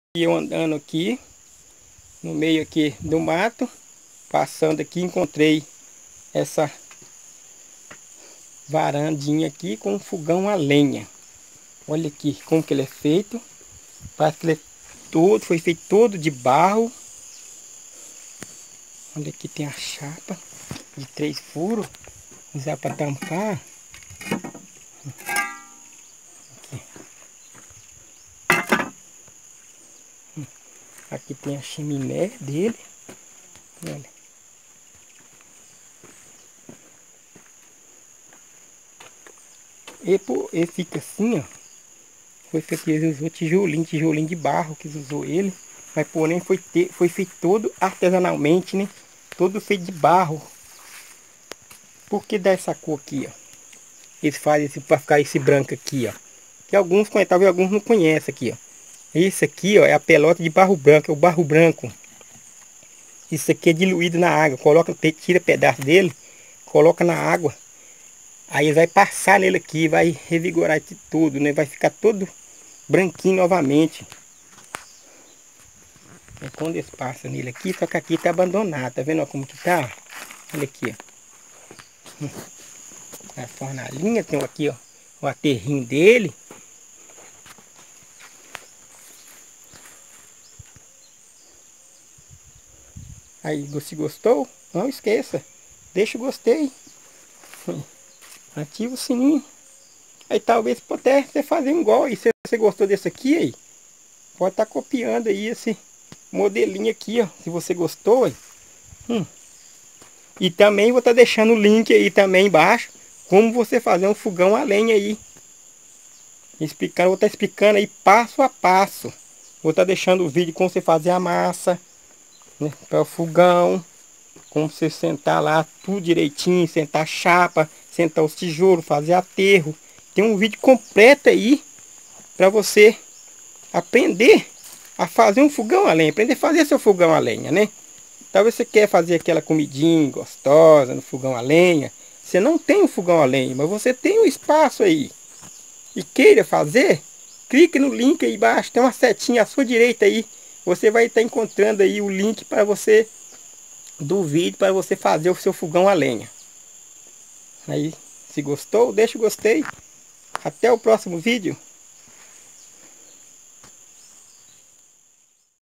Fogão a lenha é muito antigo feito de barro e tijolinho
Como fazer fogão a lenha